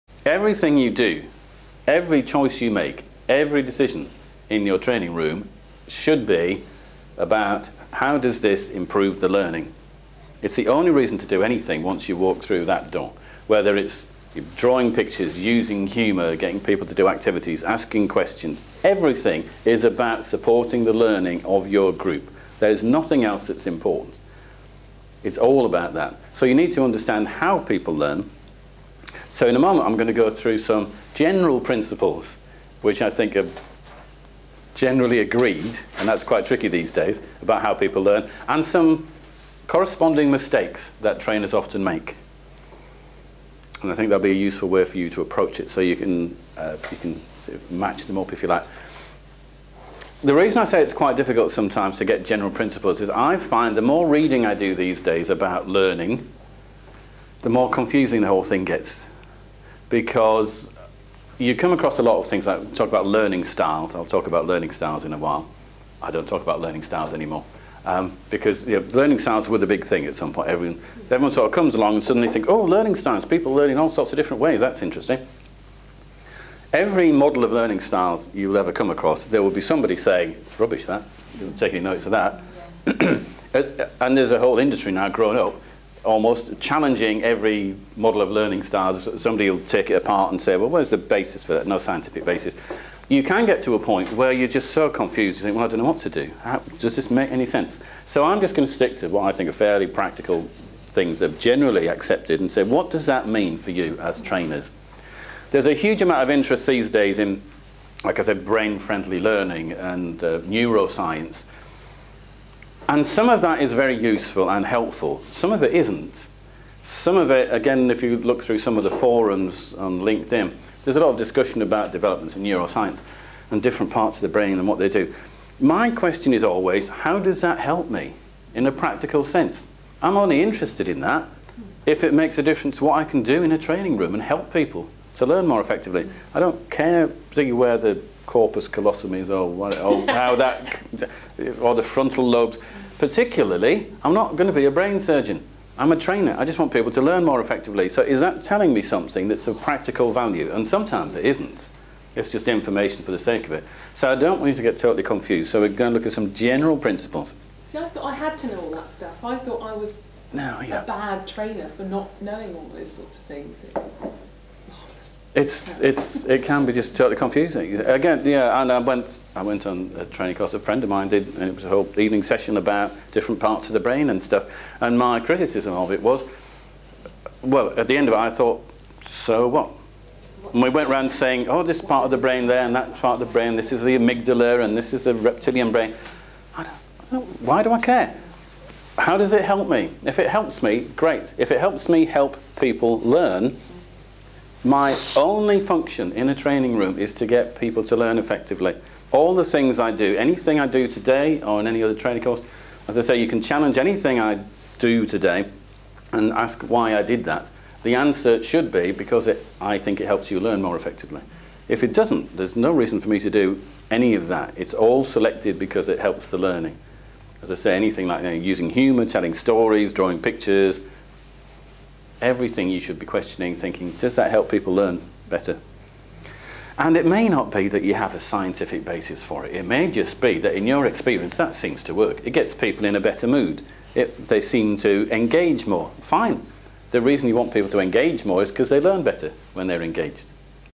Train the Trainer - Helping people to learn
Alan Matthews discusses the role of the trainer in helping people to learn effectively. This is part of his train the trainer course How To Run A Great Training Workshop. For more information on how Alan can help you and your organisation to deliver training which really makes a difference,